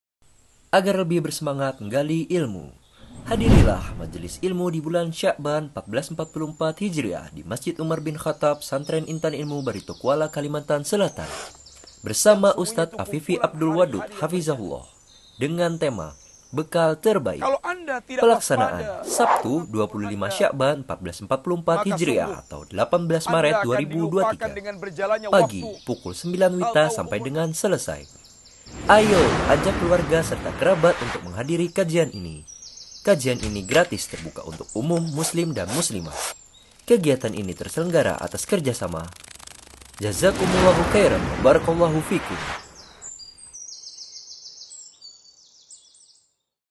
SAFARI DAKWAH DI KALIMANTAN SELATAN
Bersama :
*Ustaz Afifi Abdul Wadud, BA* _Hafizhahullahu Ta'ala_

🗓️ SABTU, 25 SYAKBAN 1444 H / 18 MARET 2023
🕛 PAGI Pukul 09.00 WITA - Selesai
📚 KAJIAN TAUHID : BEKAL TERBAIK
🕌 Masjid Umar bin Khattab Intan Ilmu Barito Kuala

Silakan bagikan informasi ini semoga bermanfaat, Jazakumullahu Khairan wa Barakallahu Fiikum